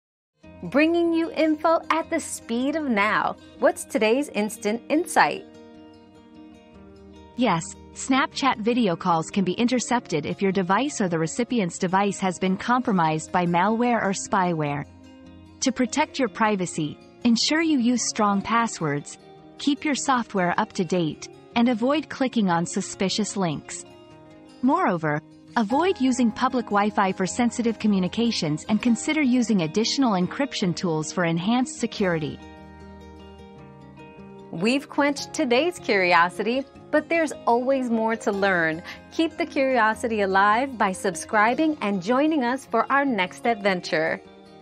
Can someone intercept your Snapchat video calls?
Is Snapchat Video Call Privacy at Risk? 👉 Snapchat Privacy Risks 👉 Learn how Snapchat video calls can be intercepted and what steps you can take to protect your privacy and enhance your security online.

Laura S. Harris (2024, June 11.) Can someone intercept your Snapchat video calls?
    🌐 AskAbout.video